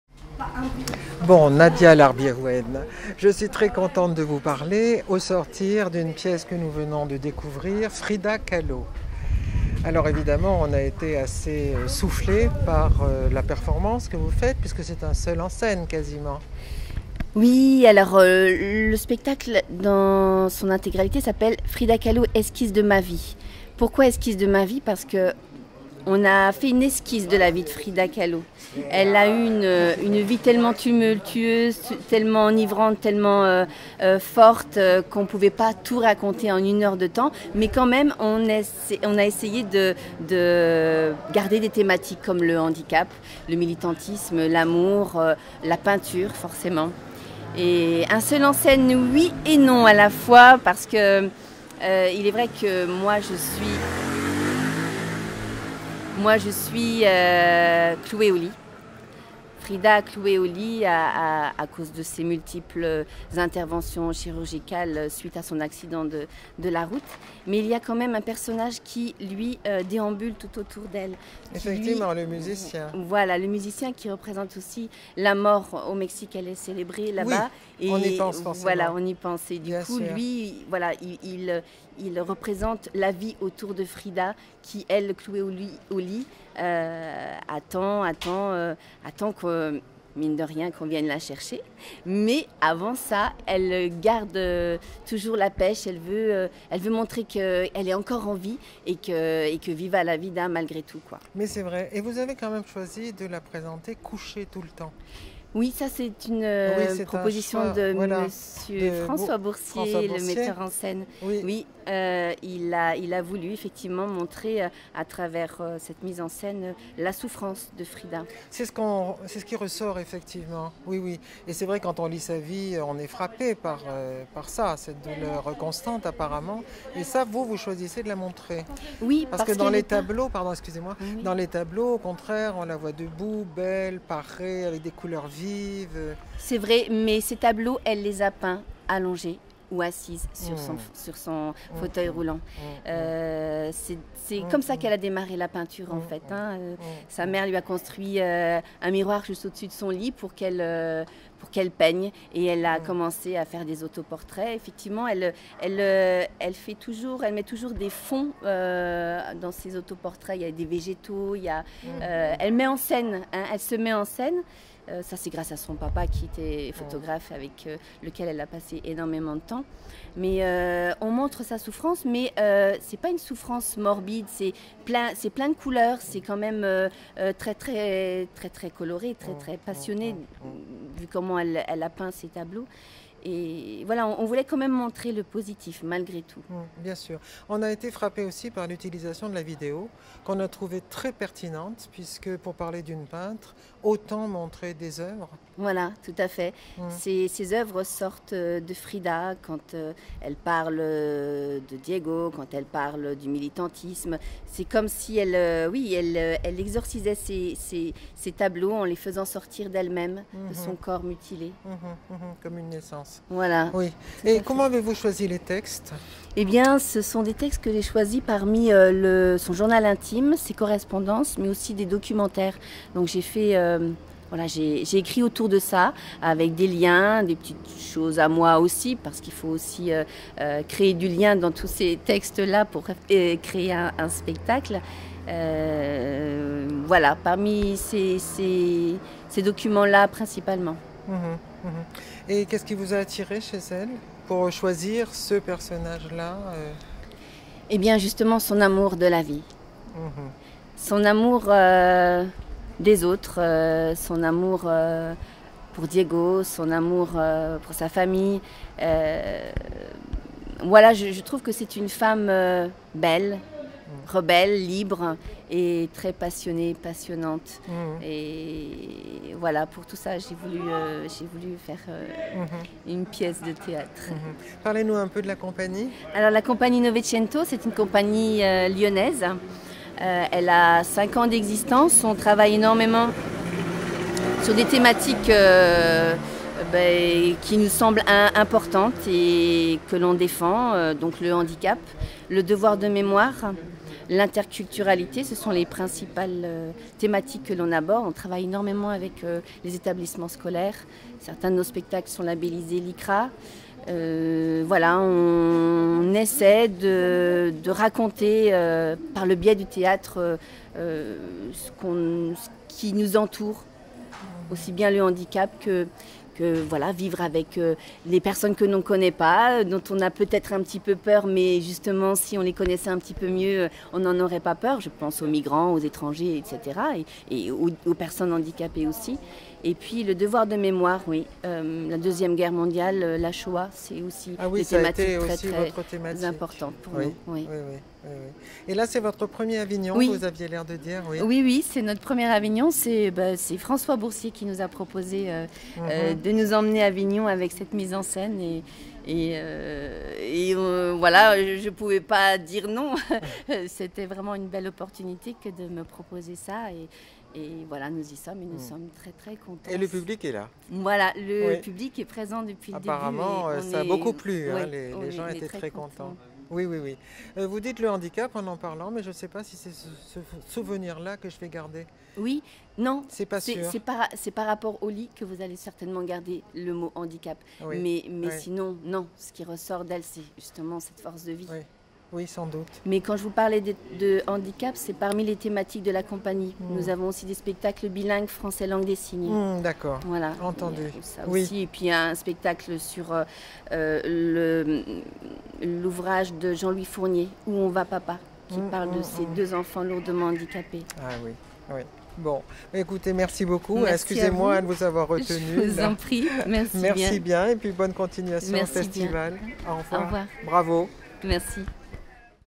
FRIDA KAHLO - Entretien avec Nadia Larbiouene
17 juillet 2018 : Entretien avec Nadia Larbiouene

Frida Kahlo, esquisse de ma vie, au théâtre des Barriques, mise en scène François Bourcier.
Interprète : Nadia Larbiouene
OFF 2018 - Avignon

Interview réalisée par Isabelle Royer, présidente de l'association MCH - Maison de la Culture du Havre.